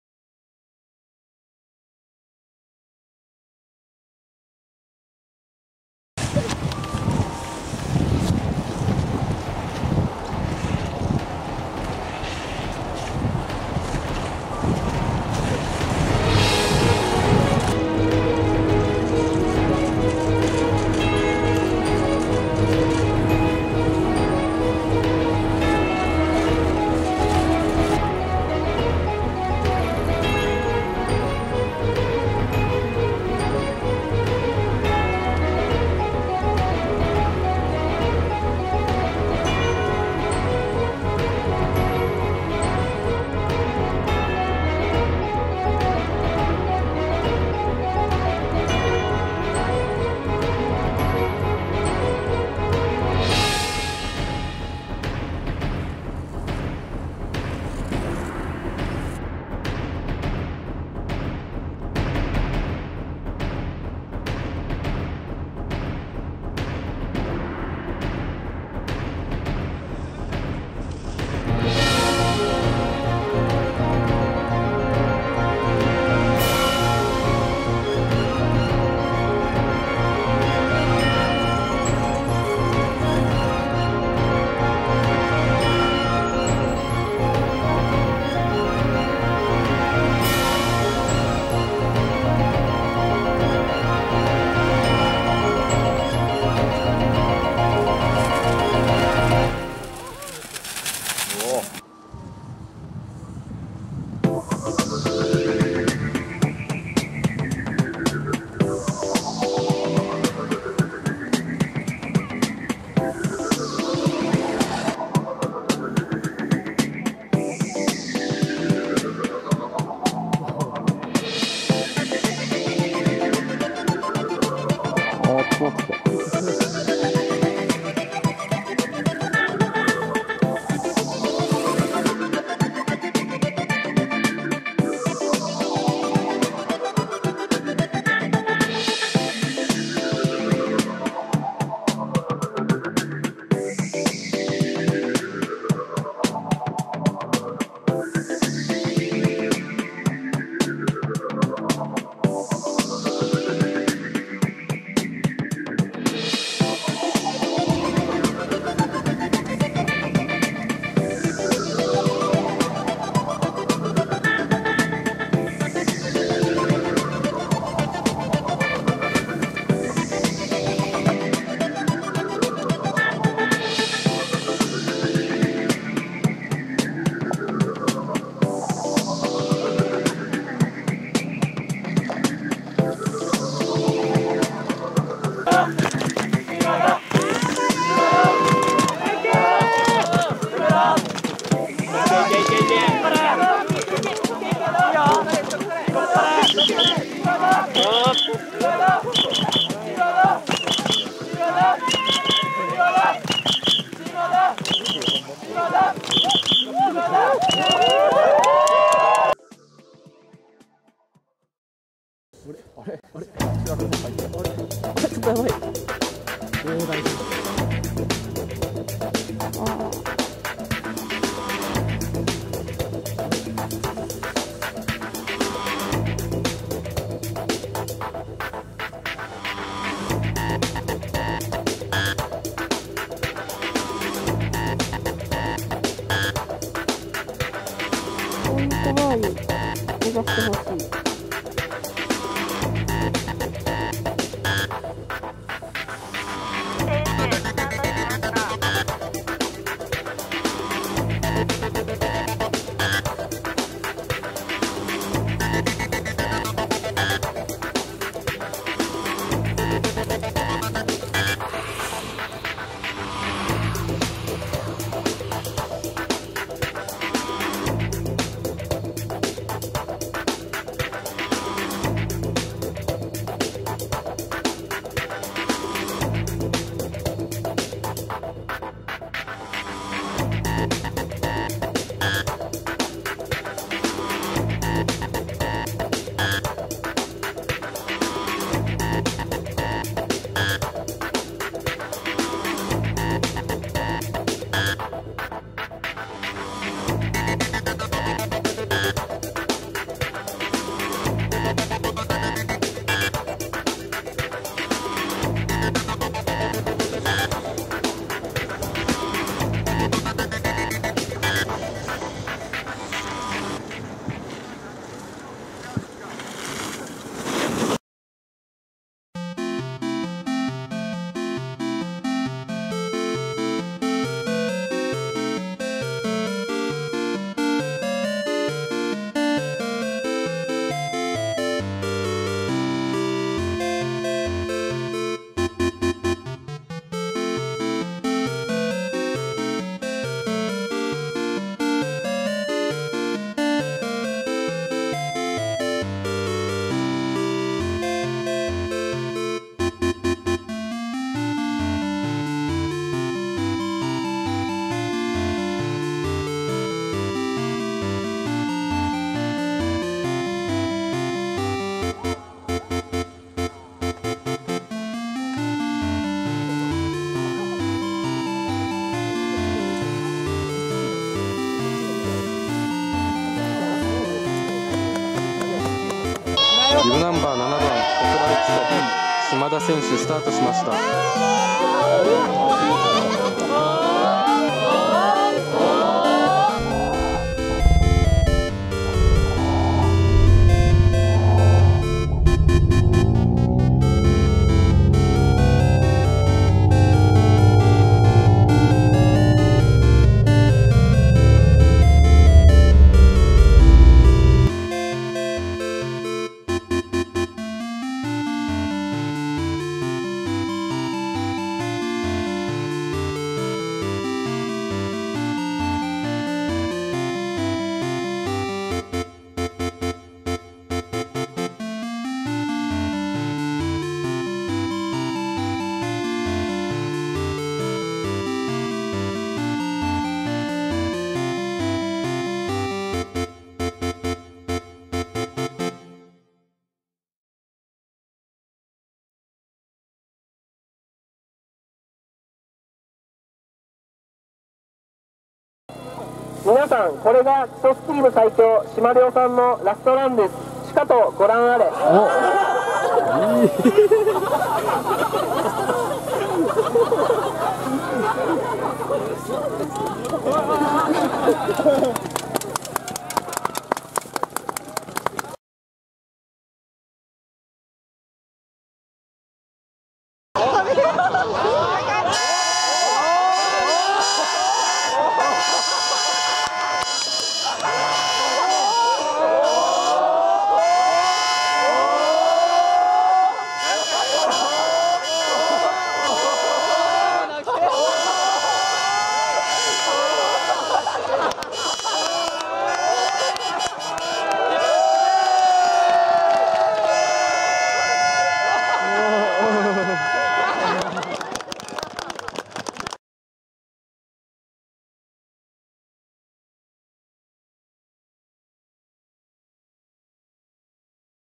【学生6年間の成長記録】どのスポーツも大成しなかった僕としては上出来だったのでは
しまりょーチャンネルno6
学生スキーが終わりました。
これからは社会人スキーです。

今まで支えてくださった方々に感謝申し上げます。
そしてこれから関わる多くの方々、どうぞよろしくお願い申し上げます。